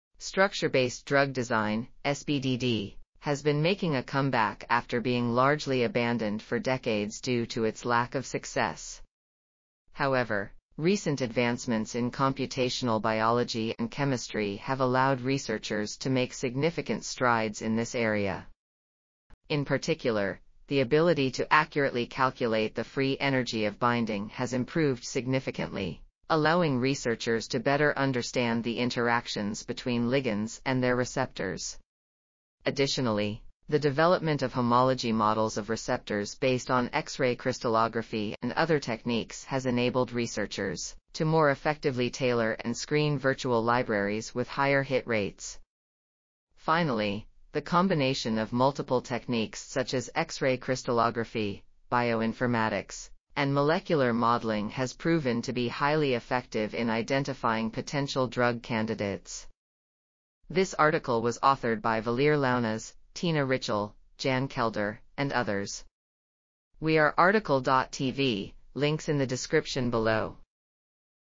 CURRENT PROGRESS IN STRUCTURE-BASED RATIONAL DRUG DESIGN MARKS A NEW MINDSET IN DRUG ... | RTCL.TV
### Keywords ###

### Article Attribution ###
Title: CURRENT PROGRESS IN STRUCTURE-BASED RATIONAL DRUG DESIGN MARKS A NEW MINDSET IN DRUG DISCOVERY
Authors: Valère Lounnas, Tina Ritschel, Jan Kelder, Ross McGuire, Robert P. Bywater ,and Nicolas Foloppe
Publisher: Elsevier
DOI: 10.5936/csbj.201302011

### Video Timestamps ###
0:00:00 - Summary
0:01:00 - Title
0:01:06 - Outro
0:01:11 - End